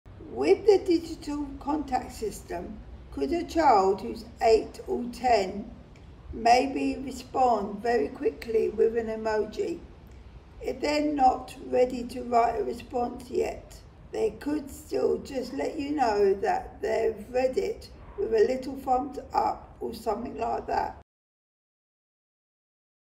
Patricia, birth parent, on the new digital contact system
Birth parents Karla, Amanda, Patricia, Adele and Sammy reflect on their experiences of traditional letterbox contact, and how they hope it might be improved through digital innovation.

This is part of our recent consultation into modernising post-adoption contact, which explores how digital solutions can address difficulties of letterbox contact.